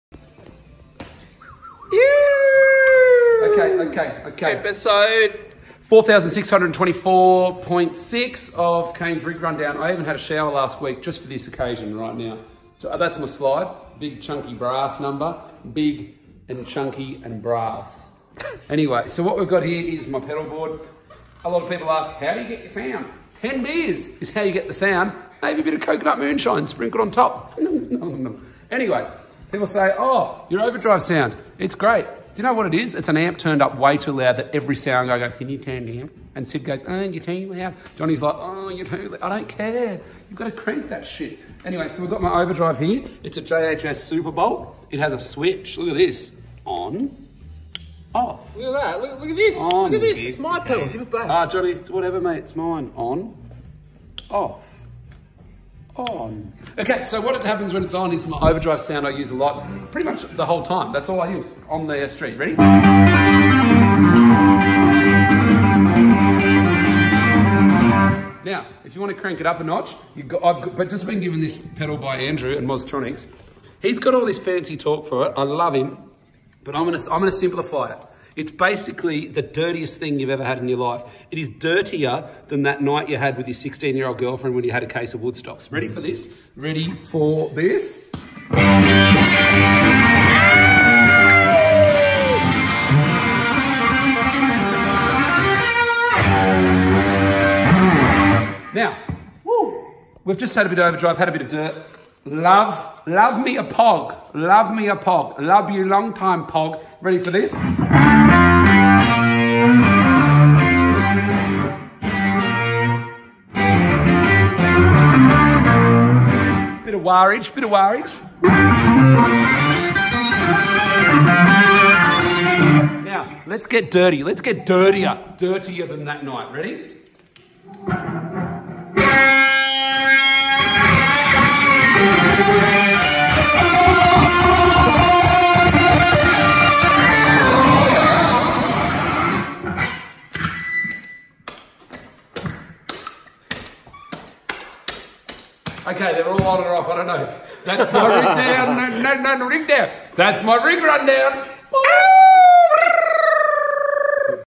Rig Rundown - Kane Dennelly - 19-Twenty
Check out, front man of 19-Twenty, Kane Dennelly’s Rig Rundown.